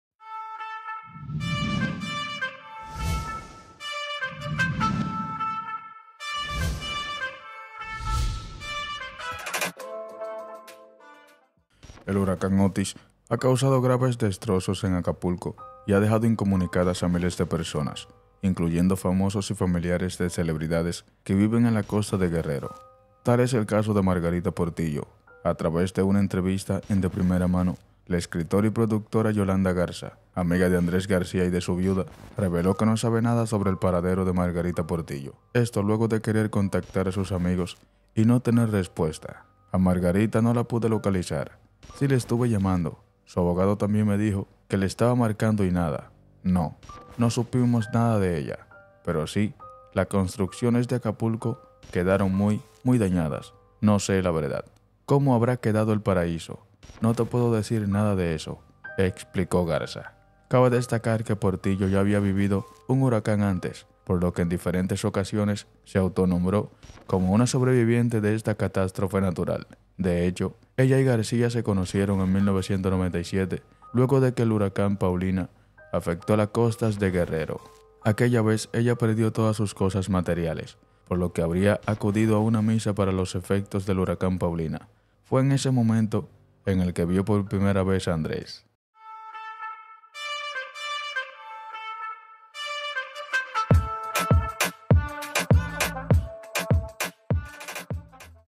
Huracán Otis: Reportan como DESAPARECIDA a Margarita Portillo, VIUDA de Andrés García
Algunos famosos y familiares de celebridades están sufriendo las consecuencias del huracán Otis.